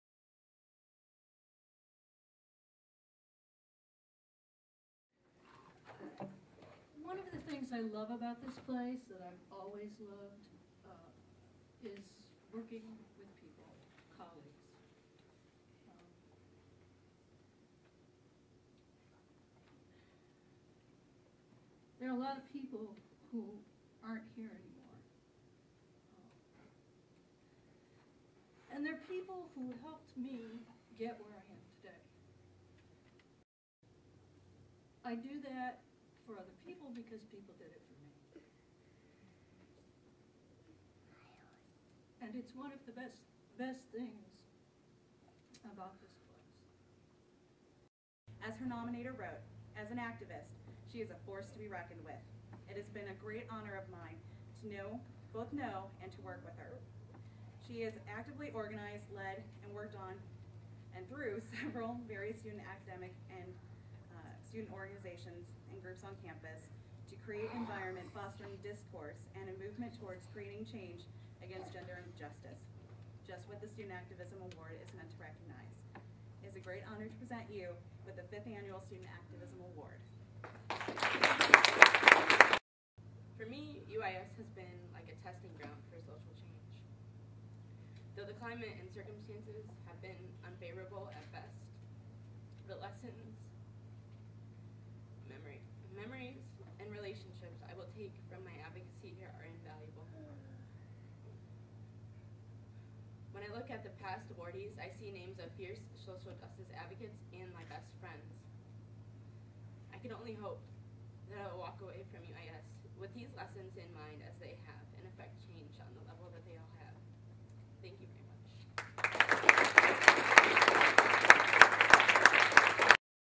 Women's Center Spring Reception at UIS
The annual Women's Center Spring Reception was held on Wednesday afternoon, April 29. Announced during the reception were the recipients of two major Women's Center awards: the 12th Naomi B. Lynn Award for Outstanding Contribution to the Environment for Women at UIS and the 5th Womens Issues Caucus Student Activism Award.